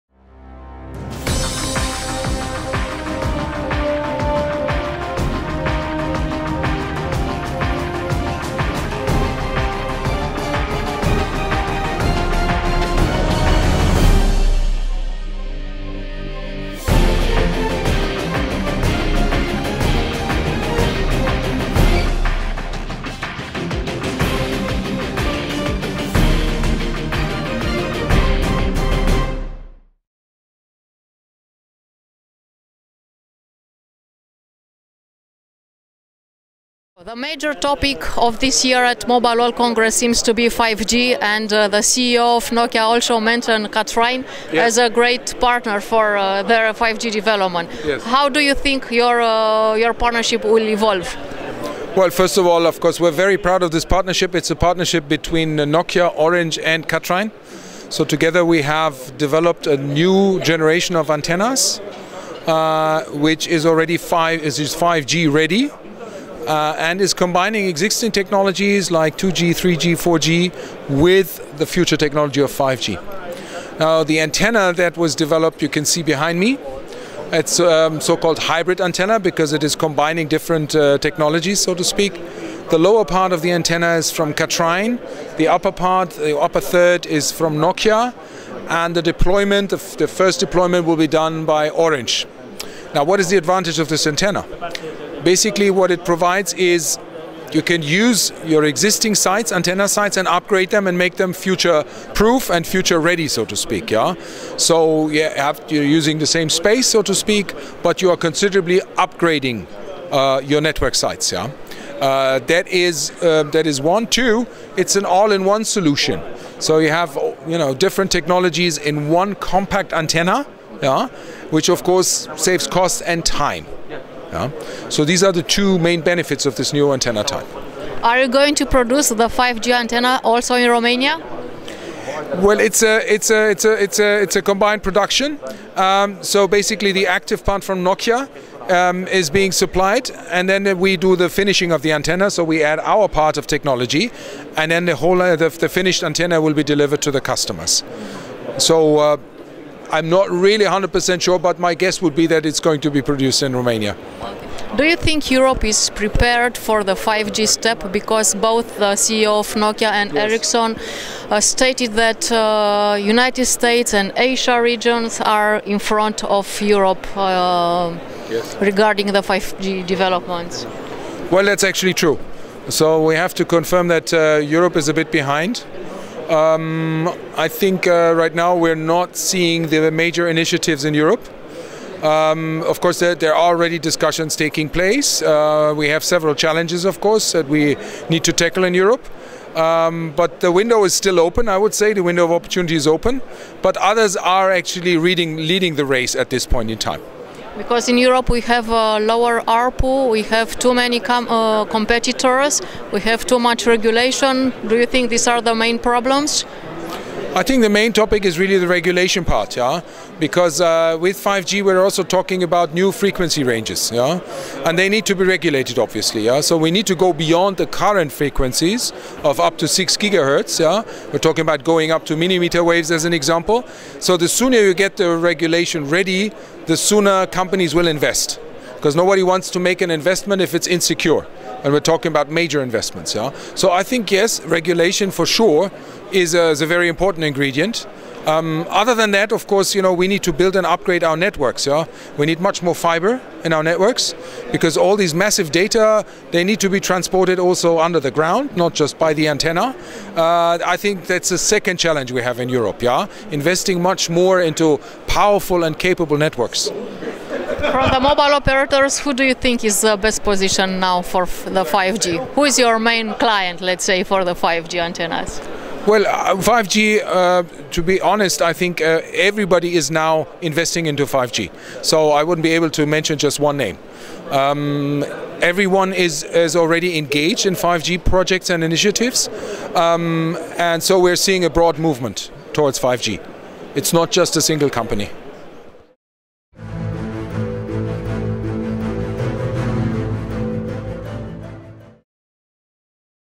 Nokia - Kathrein 5G Antenna for Orange
Frank Weber, Group VP Marketing, Kathrein
Mobile World Congress 2018, Barcelona - 26 febr - 1 mar 2018